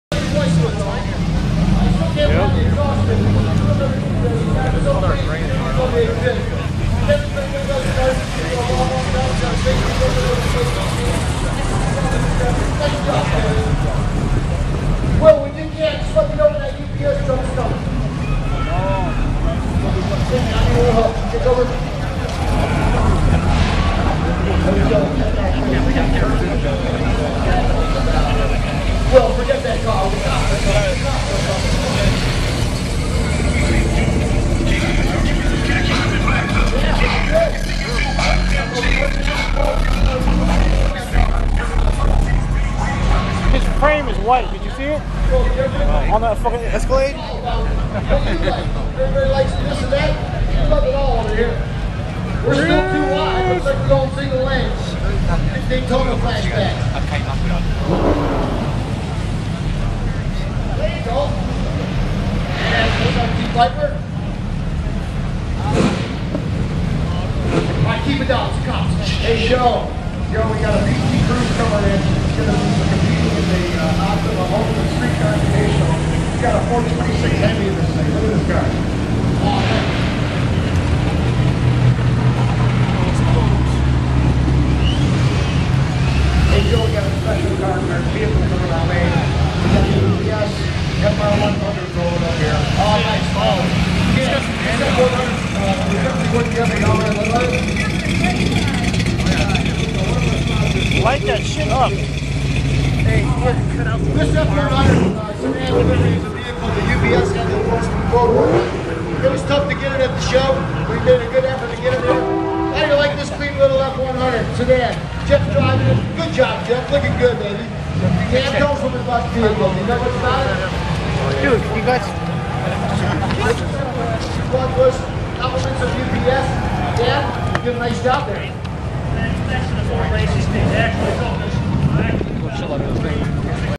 2011 Las Vegas SEMA Show Cruise Roll Out
Video of the 2011 SEMA Show Cruise Roll Out. First time SEMA did a cruise when the show ended at the end of the week. This is only some of the featured vehicles that were parked outside and nothing close to showing all the cars that were there! Enjoy this video